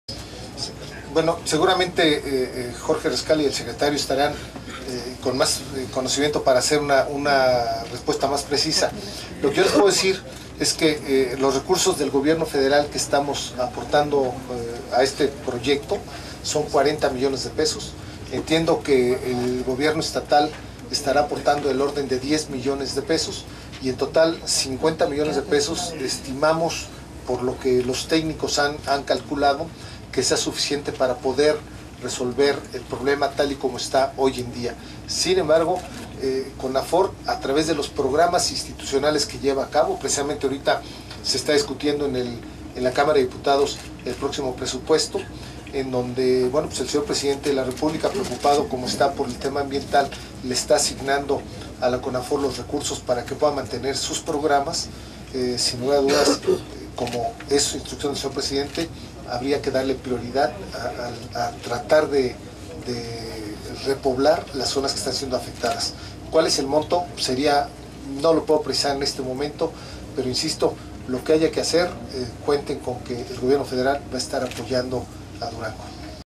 Juan Jose Guerra Abud, Suma necesaria para terminar con la plaga
Menciona de la suma que se estima será necesario invertir para terminar con la plaga la cual sera suficiente para combatir el problema.